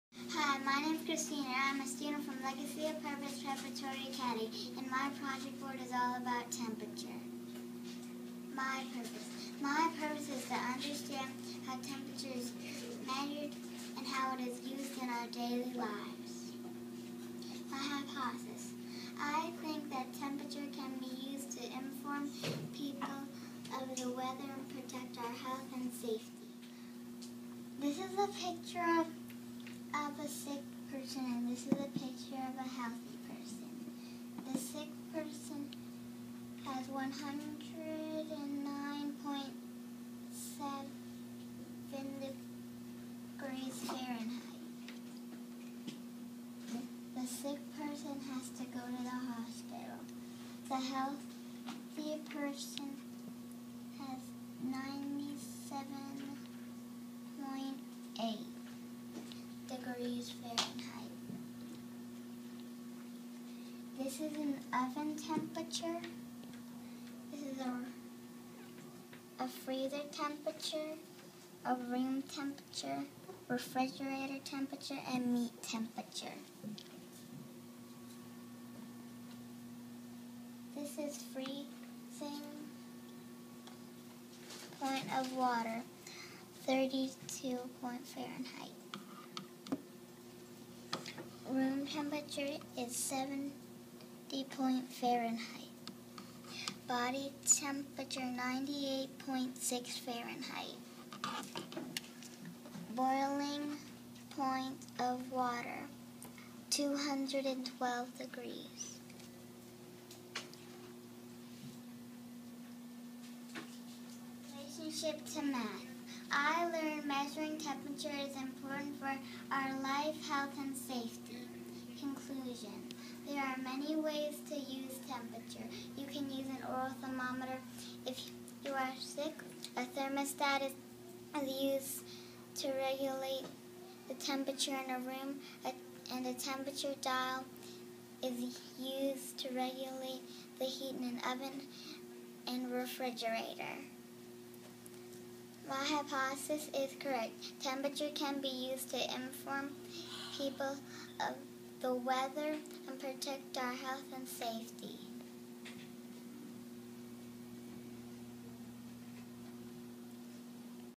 Christina's Math Project
Student at Legacy of Purpose Preparatory Academy prepares to present her Math Fair project.